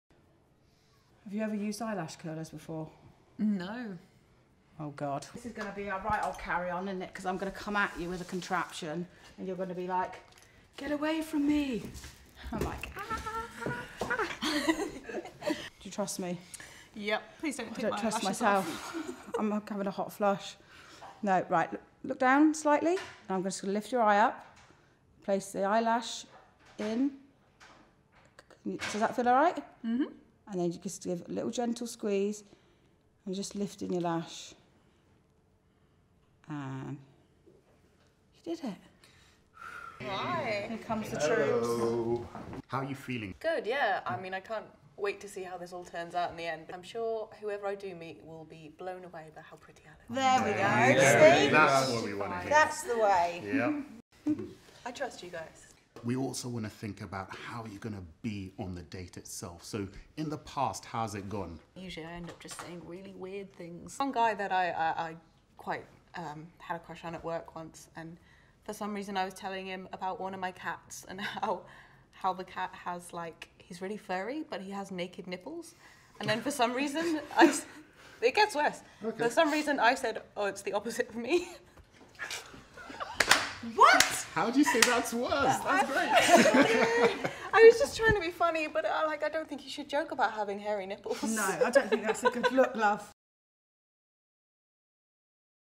Lisa Armstrong shows how to use eyelash curlers
...and what NOT to say on a first date! hairynipples!! In another taster for our new show.